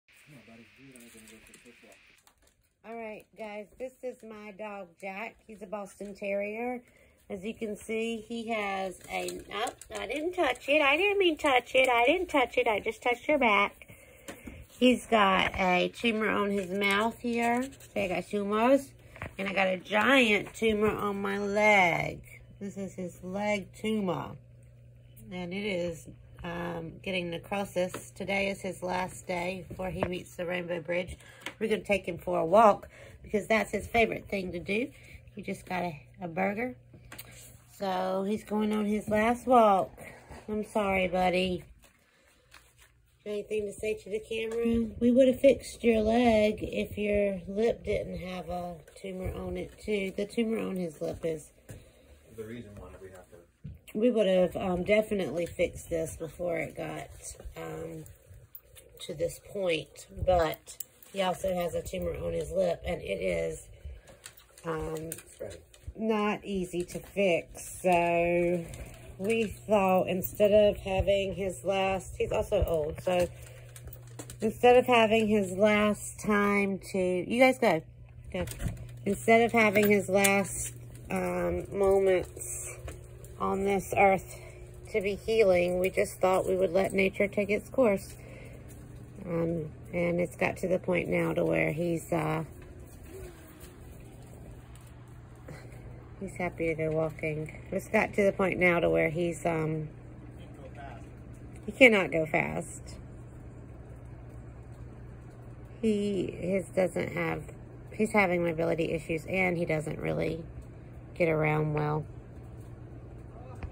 What Happens if I Ignore My Dogs Tumor - Our Beloved Boston Terrier's Last Day
This is Jack, a 13-year-old Boston Terrier. Jack had a small nonmalignant tumor pop up on his leg, which is quite common with this breed,  and before we could have it treated, he had another small tumor pop up in his mouth area. The tumor on his leg was 100% treatable, but unfortunately, the tumor in his mouth was much more aggressive than that on his leg and would require pieces of his mouth to be removed, if even that were possible. So instead of treating him for the leg tumor and having him undergo surgery we just let nature take its course, as the mouth tumor would most certainly take his life within the next year anyway. This was not an easy decision but we wanted his last little bit of time on this earth to be spent pain-free instead of undergoing surgery that might not even increase his lifespan anyway. This video was taken on his last day on this Earth and he went peacefully to the rainbow bridge about an hour later. He was a great dog and will always be loved and remembered.

Most videos are unedited and unscripted.
Any photography and editing by Amanda In Wonderland Photography